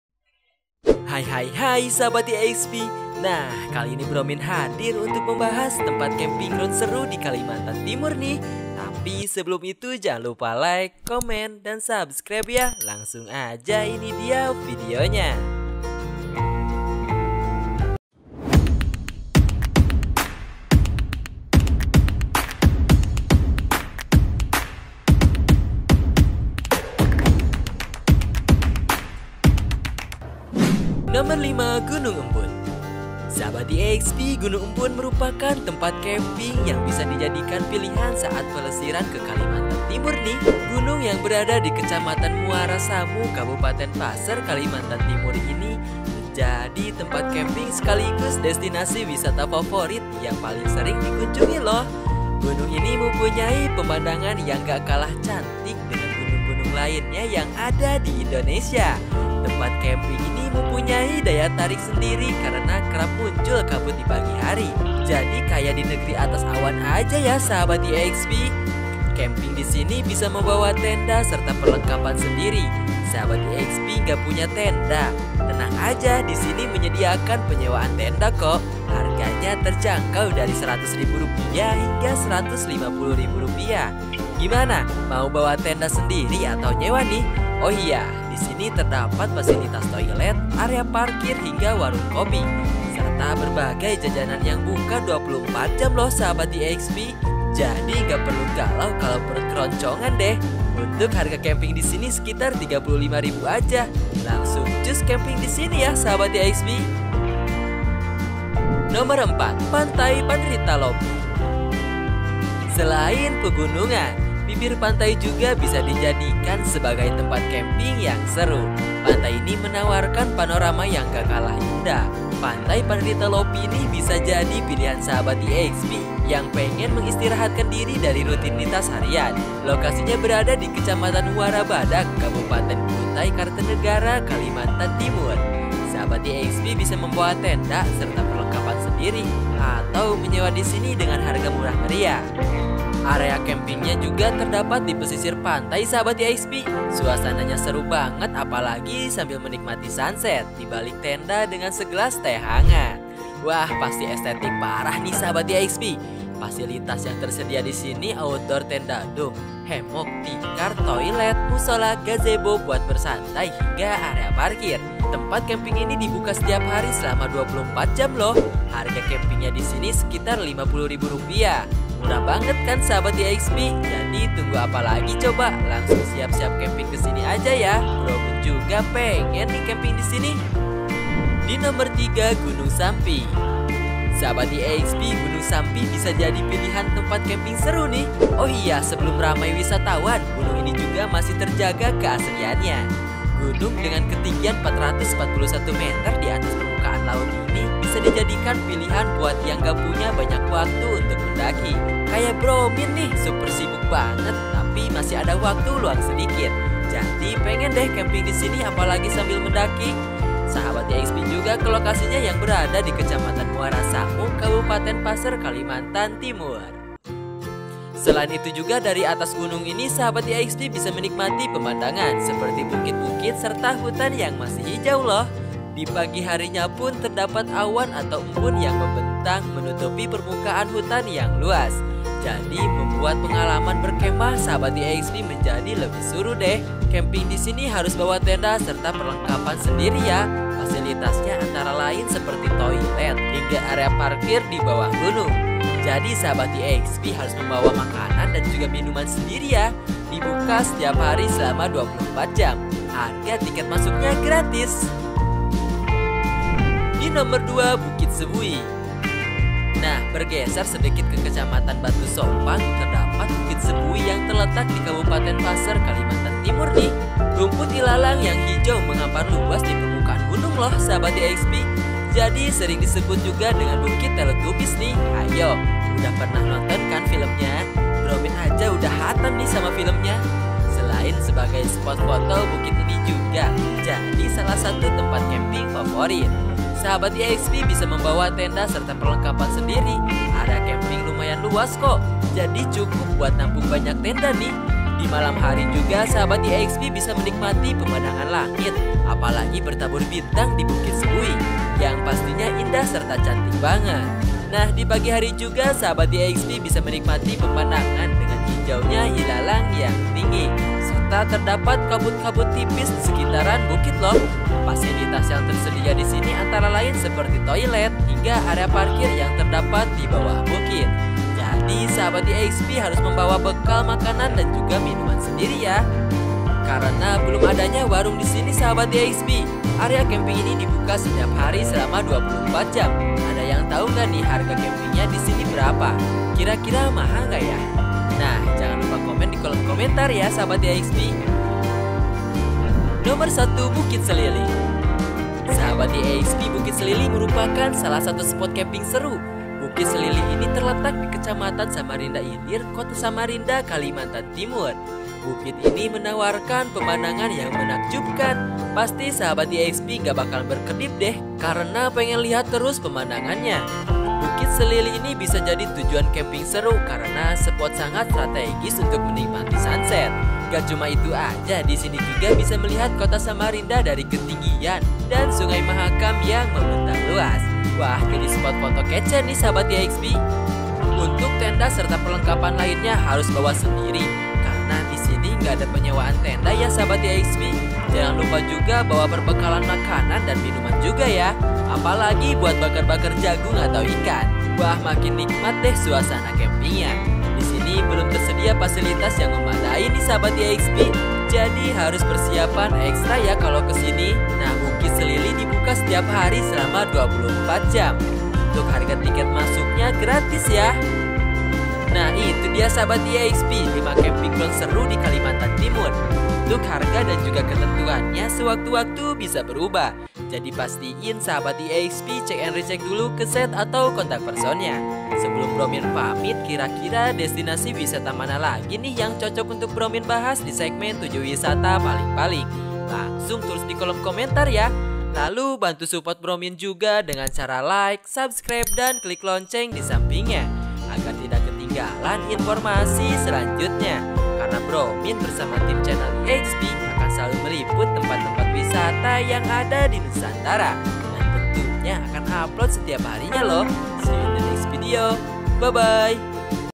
TEMPAT CAMPING GROUND SERU DI KALIMANTAN TIMUR | WISATA ALAM KALTIM COCOK DIJADIKAN CAMPING KELUARGA
Kalimantan Timur adalah sebuah provinsi Indonesia di Pulau Kalimantan bagian ujung timur yang berbatasan dengan Malaysia, Kalimantan Utara, Kalimantan Tengah, Kalimantan Selatan, Kalimantan Barat, dan Sulawesi.

Dengan banyaknya lokasi wisata yang menyuguhkan pemandangan indah di Kalimantan Timur, tidak heran jika banyak camper yang menjadikan Kalimantan Timur sebagai tujuan liburan. Sahabat Exp akan menemukan cukup banyak area – area untuk berkemah yang mengesankan. Kalimantan Timur sendiri dapat menjadi alternatif yang sangat baik bagi para camper yang sedang mencari area untuk berkemah di luar Pulau Jawa.

Semoga dengan harapan besar video ini bisa membantu Sahabat Exp untuk mencari temapat camping yang rekomen...